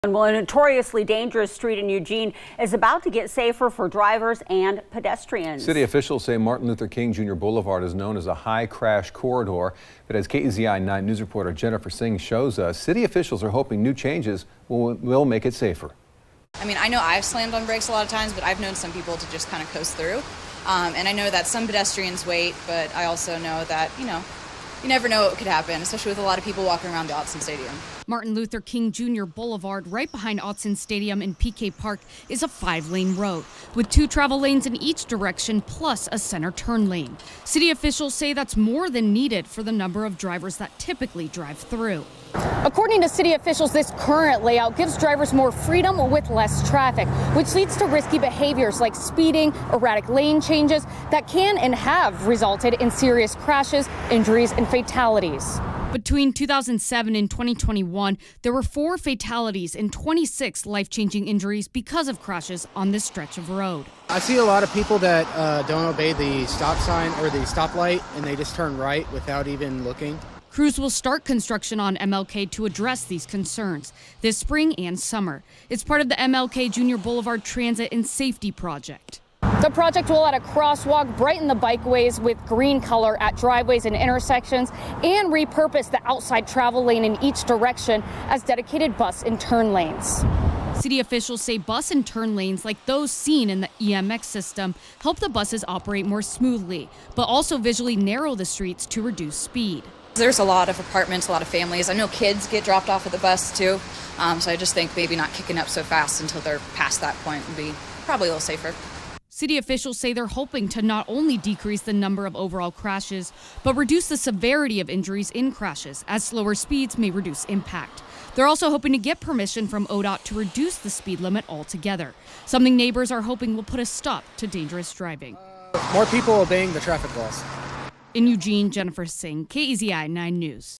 MLK Jr. Blvd. undergoing lane changes to promote safer driving habits
According to the City of Eugene, the current lane configuration offers more capacity than necessary.